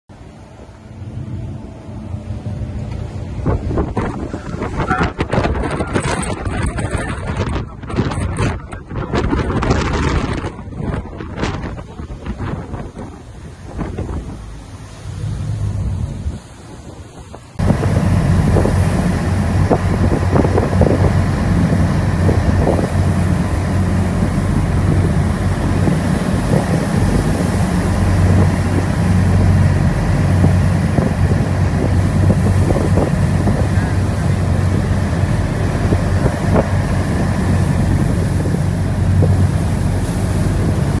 Three Irish RNLI Lifeboats Aid Disabled Cargo Ship in Heavy Seas
Crews from Rosslare Harbour, Kilmore Quay and Dunmore East battle large waves to save the powerless vessel Lily B off the Irish coast

See the full story on MarineLink.com.

(Video courtesy RNLI)